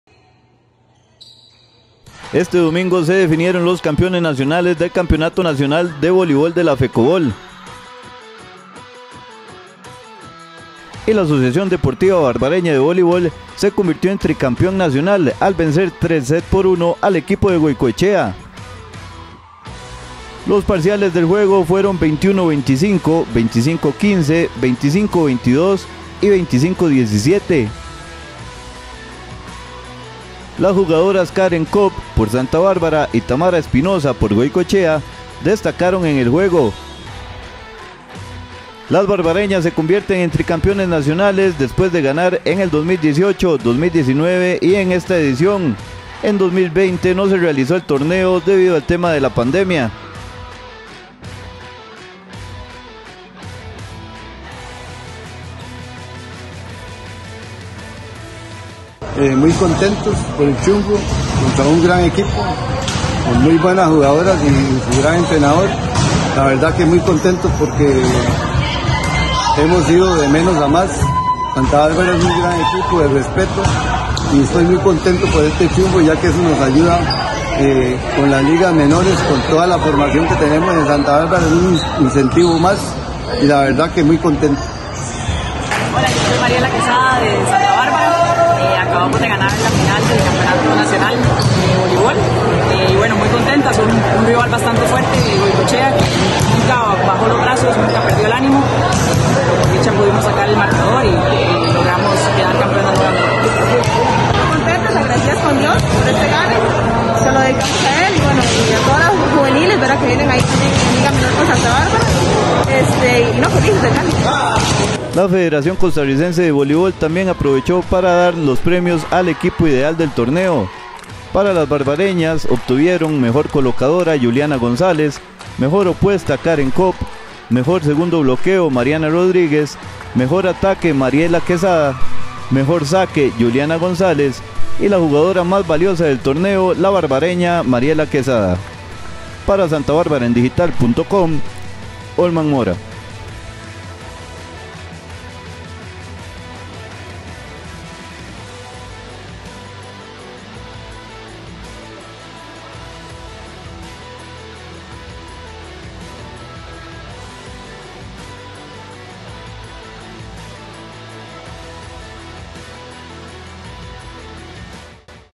Santa Bárbara es Tricampeón del Voleibol de primera división.
Las barbareñas vencieron 3 set a 1 a Goicoechea en la final del Campeonato Nacional de Primera División de voleibol de la FECOVOL.